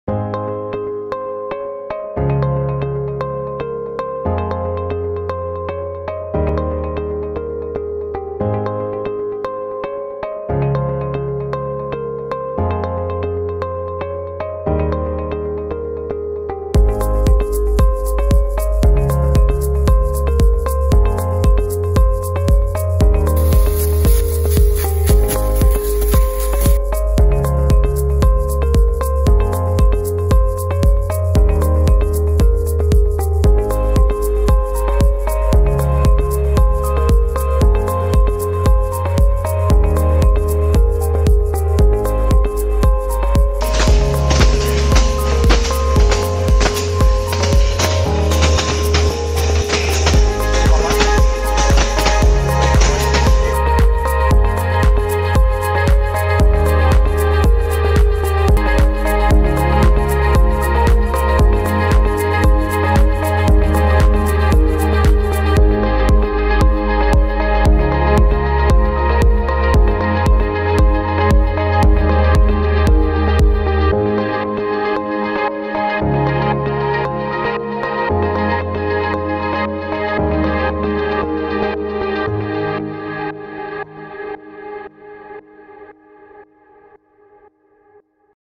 Scorpion Vision Post Harvest Vegetable Processing
In summer 2020 we entered the British Farming Awards with our post harvest vegetable processing 3D robot vision system.